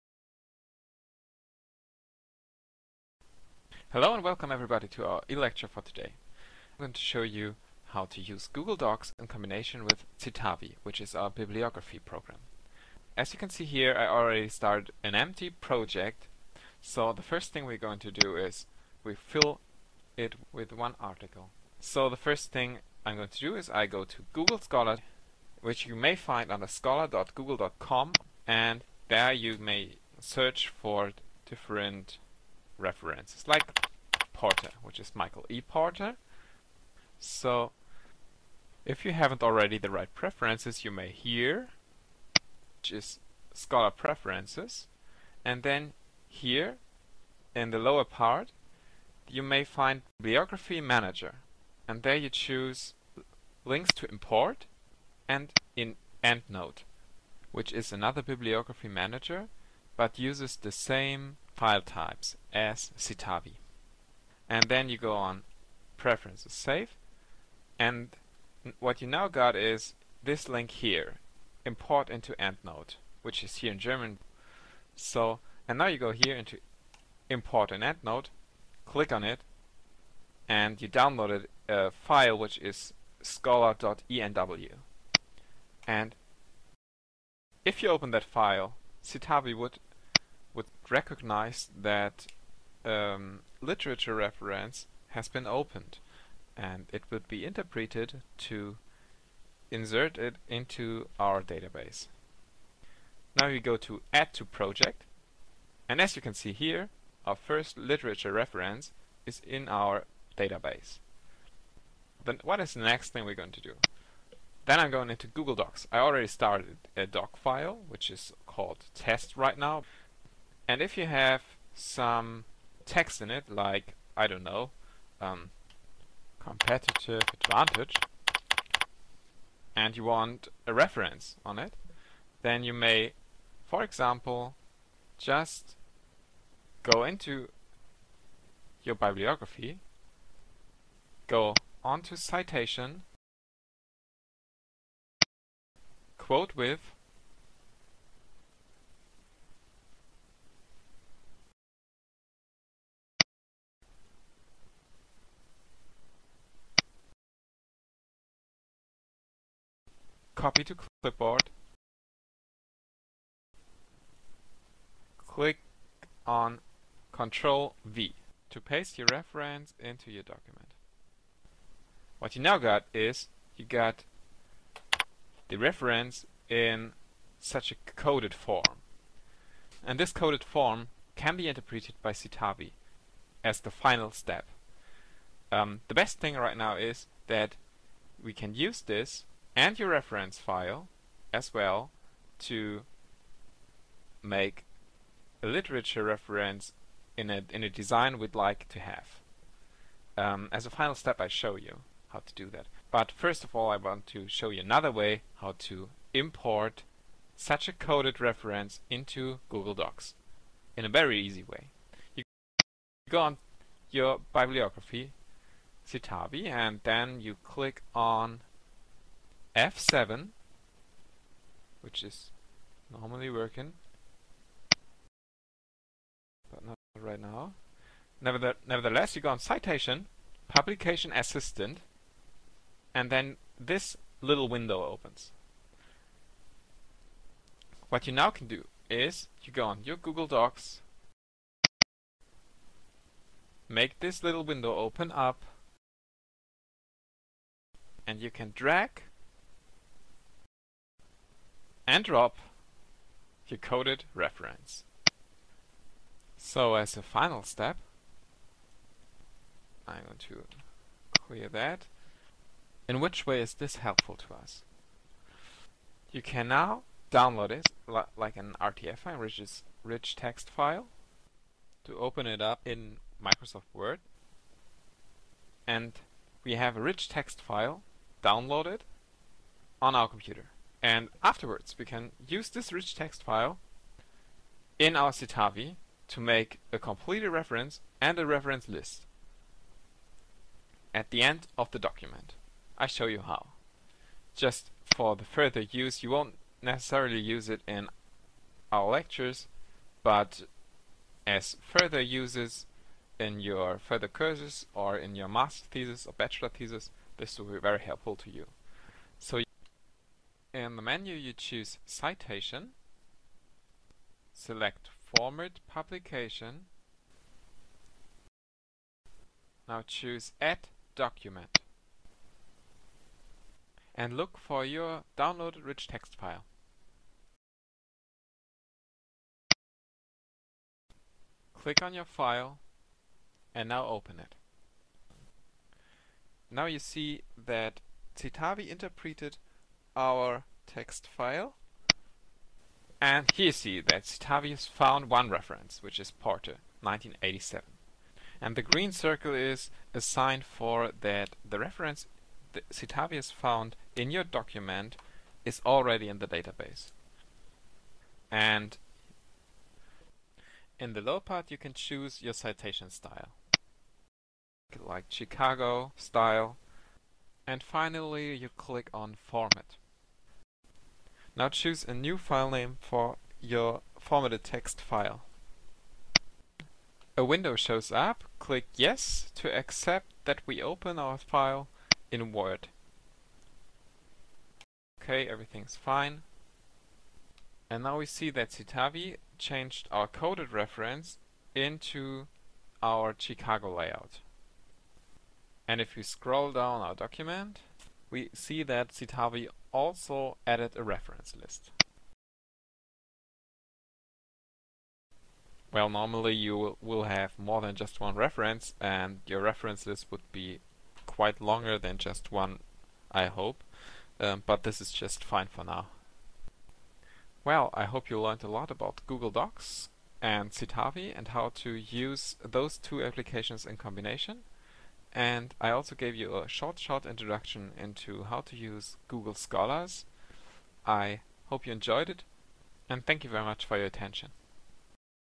Tutorial for Google Docs and Citavi
This video-tutorial introduces usage of Google Docs in combination with bibliography software Citavi.
Do you want to work collaboratively on a scientific article, but distance between your locations is to big? This tutorial gives you a fair amount of hints how to solve these issues.